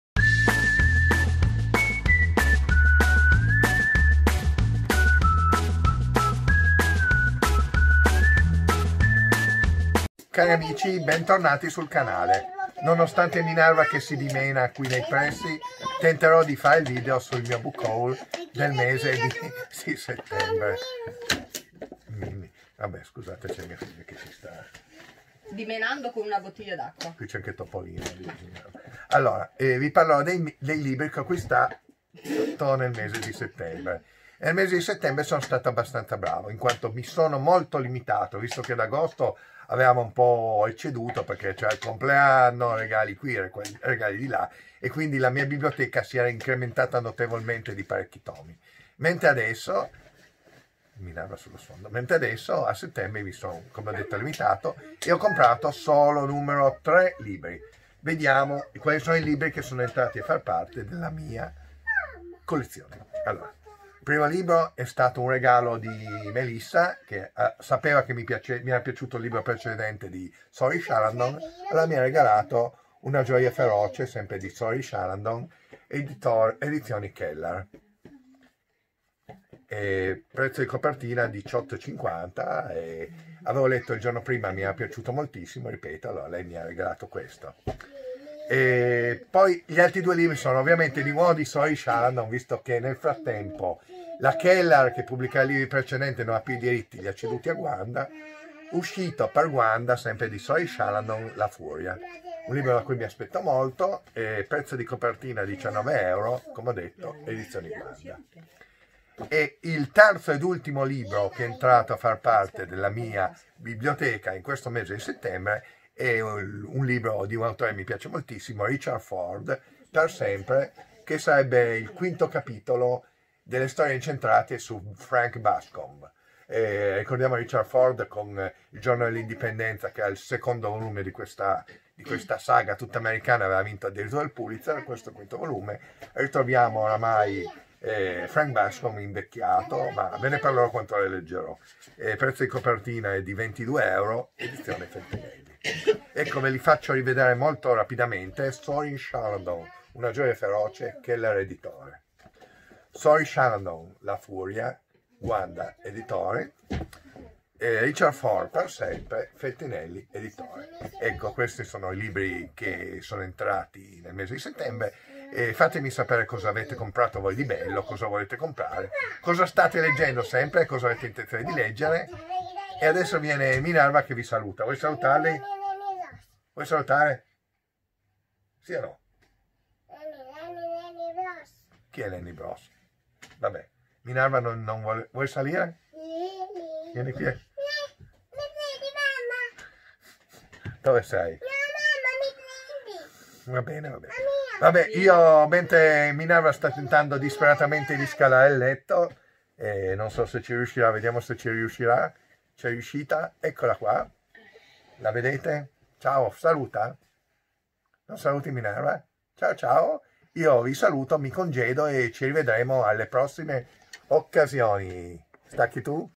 Book Haul Settembre 2024 Gio
Buonsalve, gente!
Oggi tocca a Giorgino parlarvi dei suoi contenuti acquisti settembrini :D